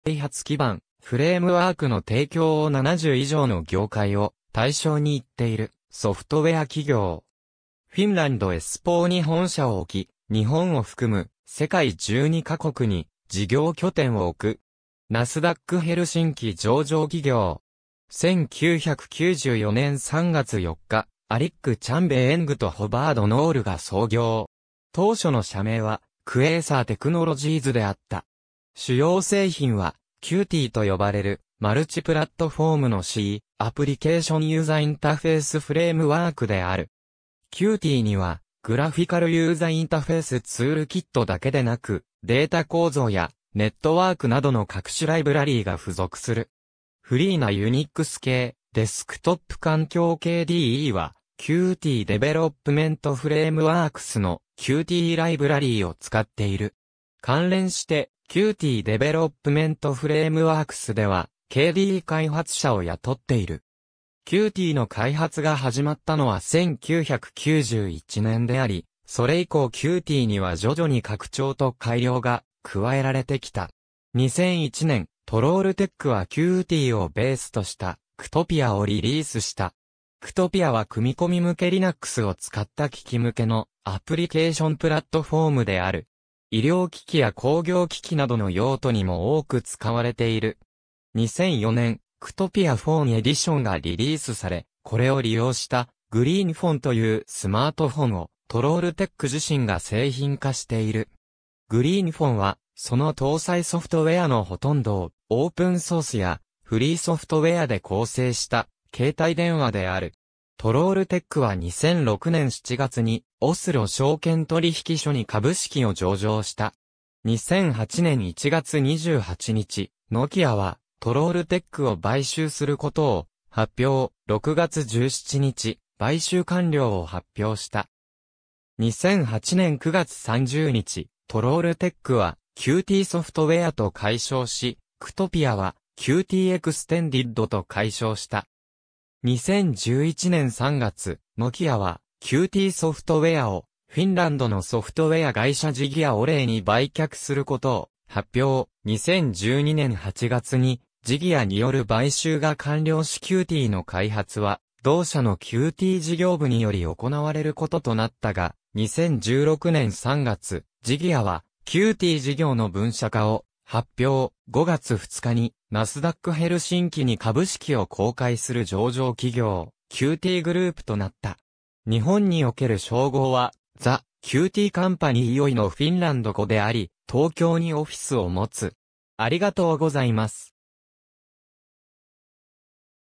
The Qt Company
The Qt Companyは、ソフトウェア開発基盤/フレームワークの提供を70以上の業界を対象に行っているソフトウェア企業。
フィンランド・エスポーに本社を置き（正式社名はQt Group plc）、日本を含む世界12カ国に事業拠点を置く。
ナスダック・ヘルシンキ上場企業（Nasdaq Nordic QTCOM）。
1994年3月4日、Eirik Chambe-Eng と Haavard Nord が創業。
当初の社名はQuasar Technologiesであった。
主要製品は Qt と呼ばれる、マルチプラットフォームの C++ アプリケーション・ユーザインタフェース（UI）フレームワークである。
Qt には グラフィカルユーザインタフェース（GUI） ツールキットだけでなく、データ構造やネットワークなどの各種ライブラリが付属する。
フリーなUNIX系デスクトップ環境 KDE は、Qtデベロップメントフレームワークスの Qt ライブラリを使っている。
関連してQtデベロップメントフレームワークスでは KDE 開発者を雇っている。
Qt の開発が始まったのは1991年であり、それ以降 Qt には徐々に拡張と改良が加えられてきた。
2001年、トロールテックは Qt をベースとした Qtopia をリリースした。
Qtopia は組み込み向けLinuxを使った機器（携帯電話、MP3プレイヤーなど）向けのアプリケーションプラットフォームである。
医療機器や工業機器などの用途にも多く使われている。
2004年、Qtopia Phone Edition がリリースされ、これを利用した Greenphone というスマートフォンをトロールテック自身が製品化している。
Greenphone はその搭載ソフトウェアのほとんどをオープンソースやフリーソフトウェアで構成した携帯電話である。
トロールテックは2006年7月にオスロ証券取引所に株式を上場した。
2008年1月28日、ノキアはトロールテックを買収することを発表、6月17日、買収完了を発表した。
2008年9月30日、トロールテックはQtソフトウェアと改称し、Qtopia は Qt Extended と改称した。
2011年3月、ノキアはQtソフトウェアをフィンランドのソフトウェア会社Digia Oyjに売却することを発表、2012年8月にDigiaによる買収が完了しQtの開発は同社のQt事業部により行われることとなったが、2016年3月、DigiaはQt事業の分社化を発表、5月2日にナスダック・ヘルシンキに株式を公開する上場企業QTグループ（Qt Group plc）となった。
日本における商号はThe Qt Company Oy（Oyは公開有限会社（Plc）のフィンランド語）であり、東京（新東京ビルヂング）にオフィスを持つ。